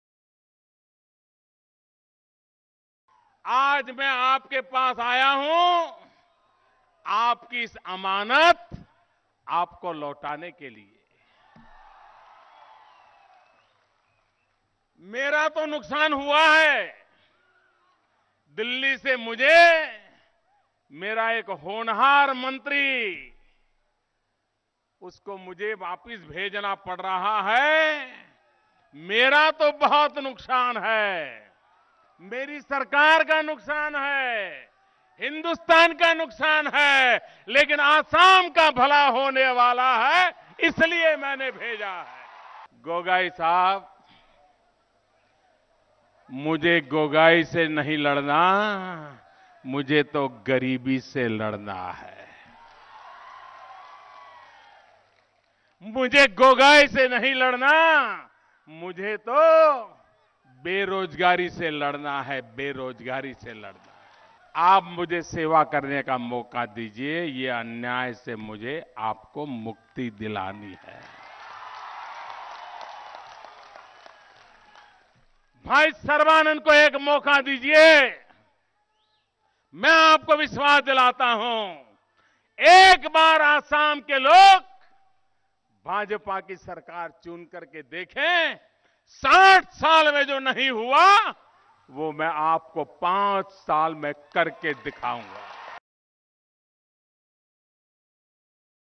Sonowal's exit loss to Centre but profitable for Assam: PM Modi
Majuli (Assam), Mar 26 (ANI): Prime Minister Narendra Modi on Saturday lauded BJP's chief ministerial candidate Sarbananda Sonowal and said that his exit from the Centre would be a big loss for the government but will be beneficial for the people of Assam. Modi, who was addressing a rally in Assam's Majuli island, said that Sonowal is the right man who can take Assam to new heights of development. The Prime Minister added that he has come to Assam to fight poverty and unemployment and not the existing Chief Minister Tarun Gogoi.  Modi further attacked Congress and said that the grand old party has not only eaten the money of poor people but have also damaged such beautiful island like Majuli.